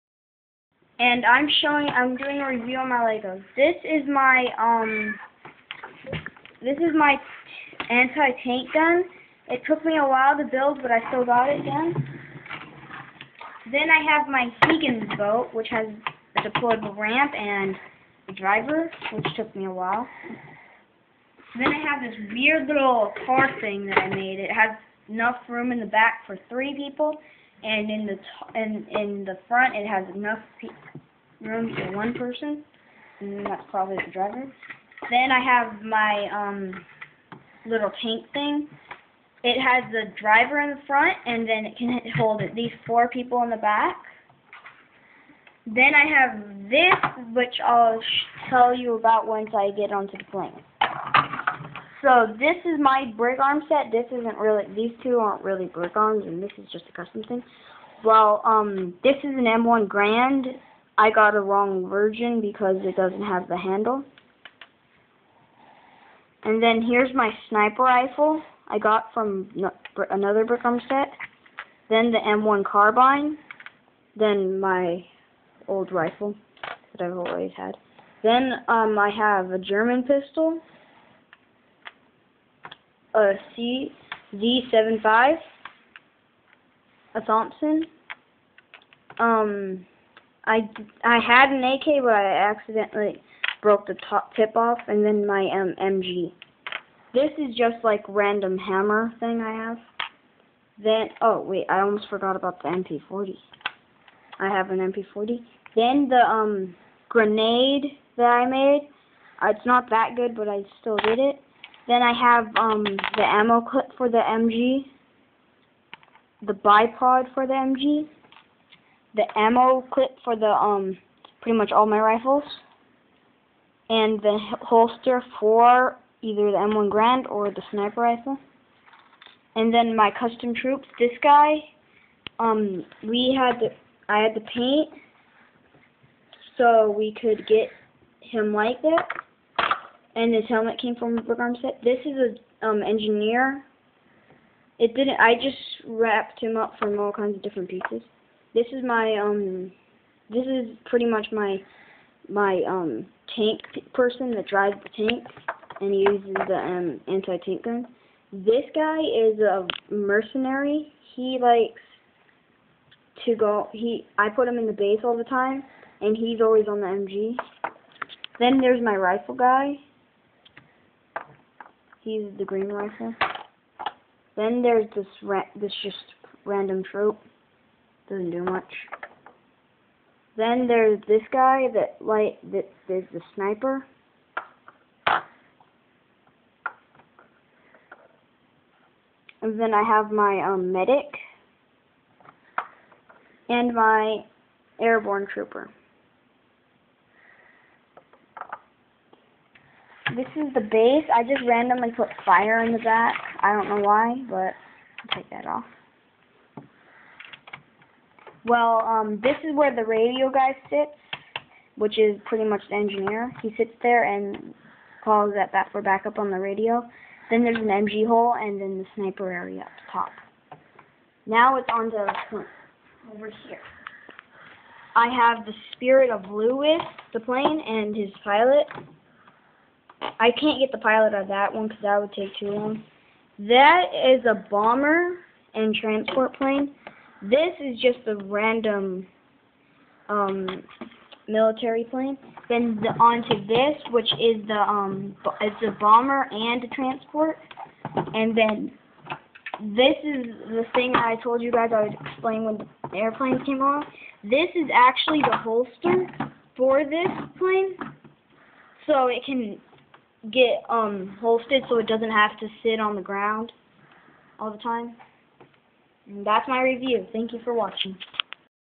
Byron cook galietti awsome Lego review
in this video i review my lego army that i made. It includes brick arms and a lot of custom made items! I hope you like this video!  please give me feedback.